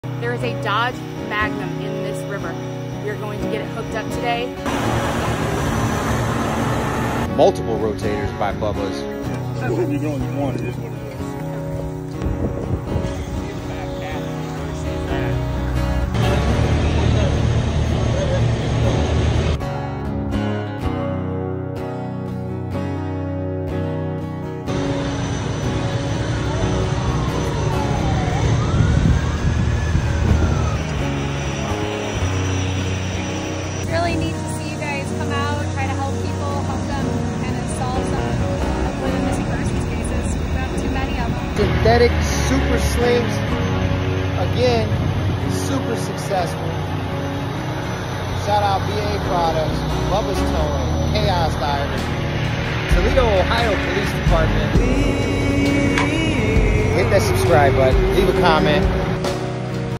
Crime Scene Recovered-Dodge Magnum Submerged
Collaborating with @ChaosDivers we were able to recover this vehicle and not only close a case but we were also able to prevent the continued toxic damage to the immediate marine environment. Huge shout out to Bubbas Towing for volunteering their time & services with multiple rotators and a total of 2 million dollars worth of equipment. The success in recoveries like this is astronomical on state and local agencies. Being able to finesse these operations completely free of charge to the government allows resources to be more appropriately dedicated to the public. Thank you Toledo Police Department, Ohio BCI, Attorney General Dave Yost & USC Missing Persons Unit & Steven Stark for all of your help. 🦾🪝